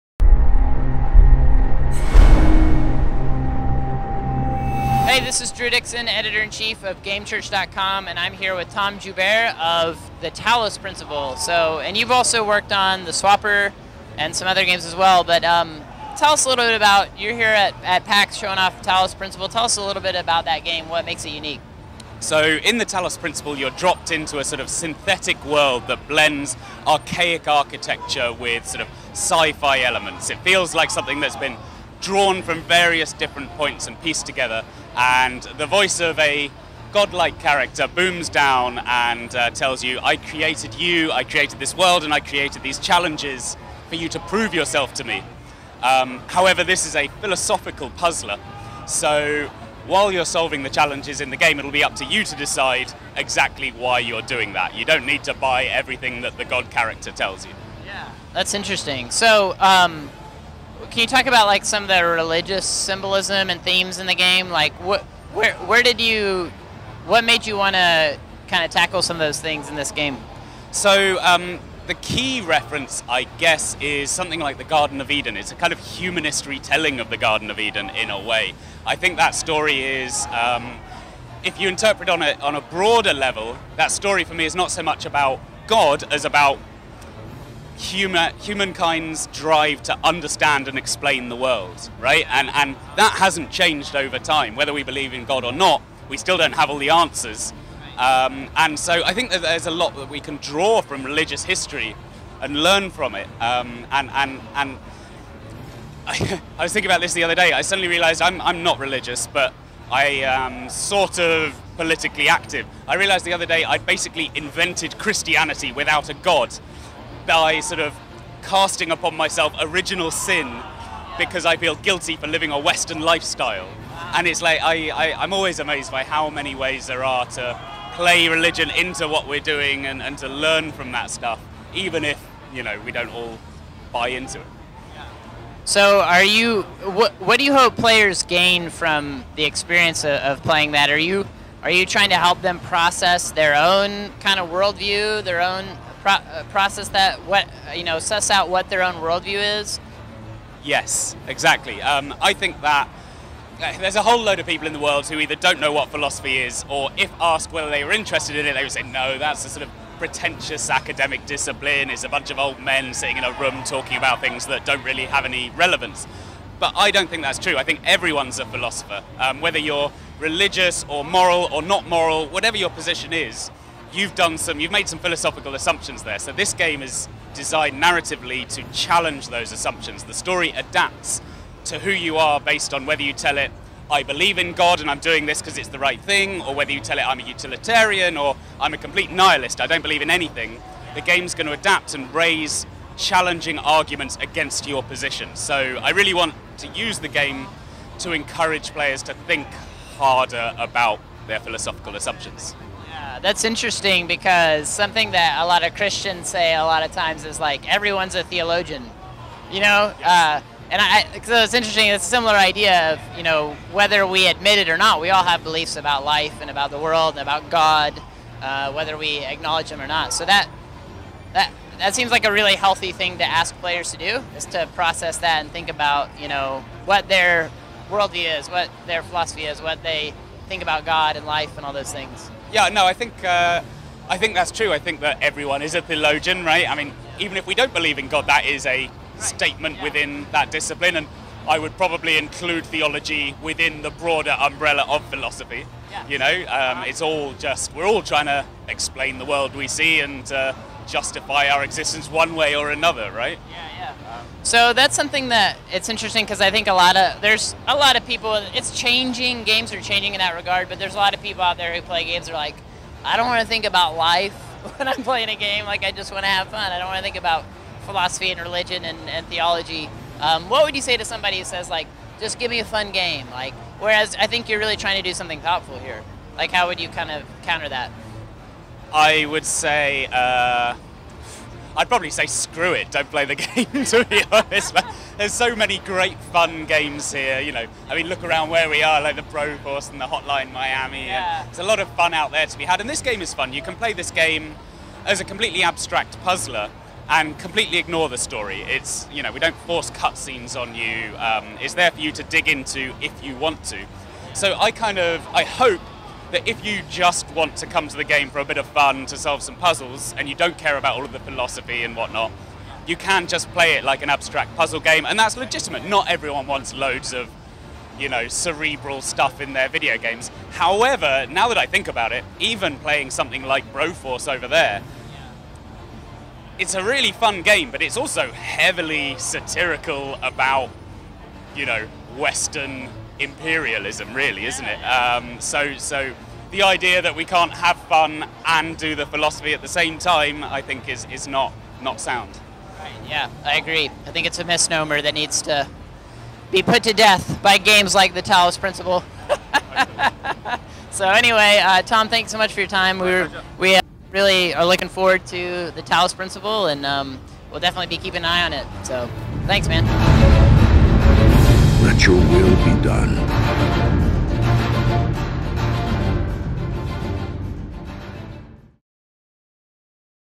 The Talos Principle Interview - PAX Prime 2014
Drew Interviews Tom Jubert about religious symbolism in the upcoming puzzler, "The Talos Principle"